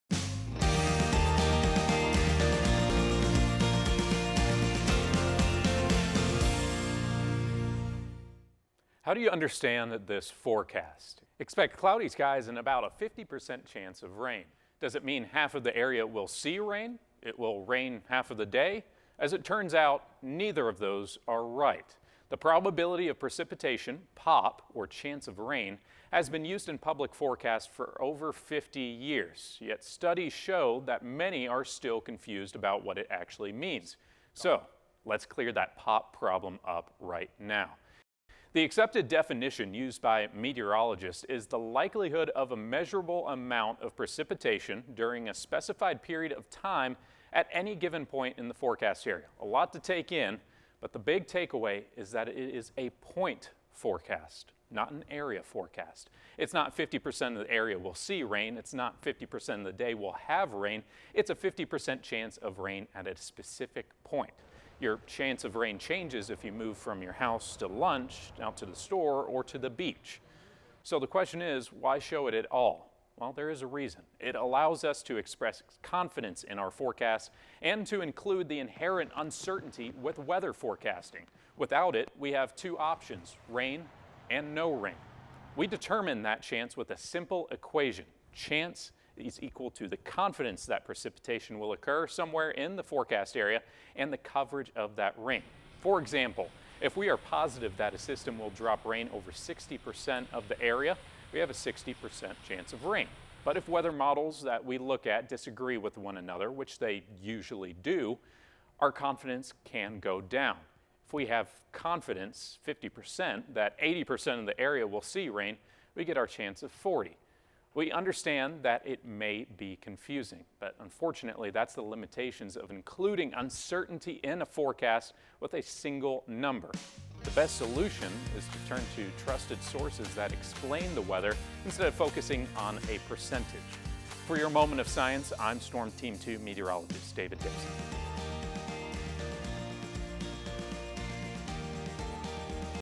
Moment of Science: What Does a 50% Chance of Rain Mean?
Episode 2:

The Probability of Precipitation (PoP) has been used in public forecasts for 50 years, yet studies show that many are still confused about what it actually means- causing confusion and misplaced distrust in meteorologists. So let’s clear that PoP problem up right now!